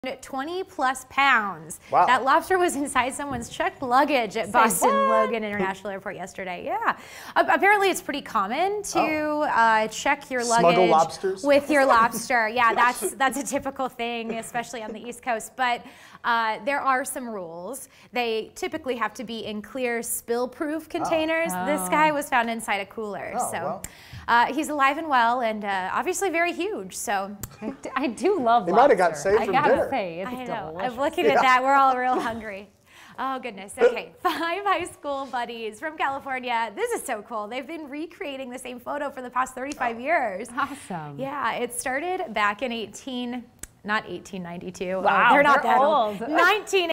Holy Sh*t -20 pound GIANT lobster smuggled in AIRPORT LUGGAGE - BUSTED!!
A T.S.A. agent with the 20-pound Atlantic lobster at Logan International Airport in Boston. Thing's huge.